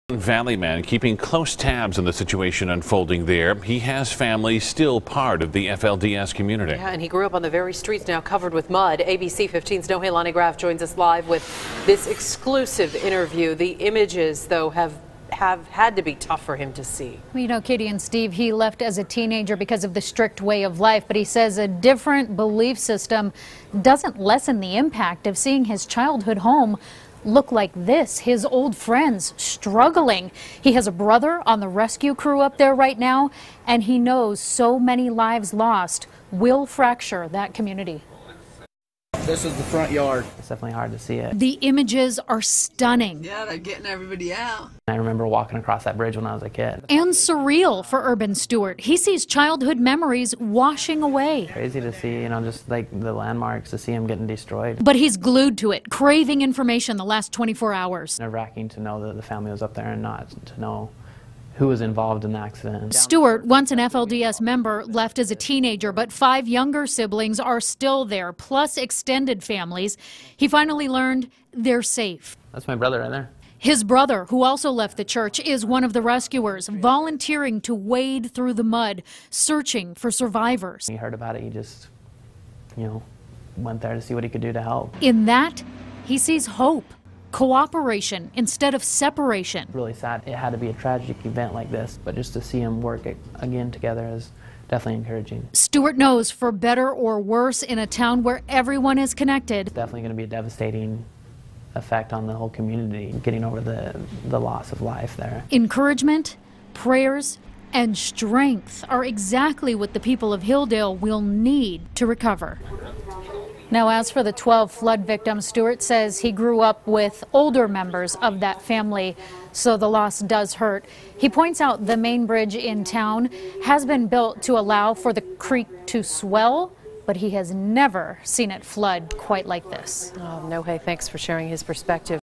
Valley man worries about family in flood zone.
Deadly flooding heartbreaking to former FLDS member.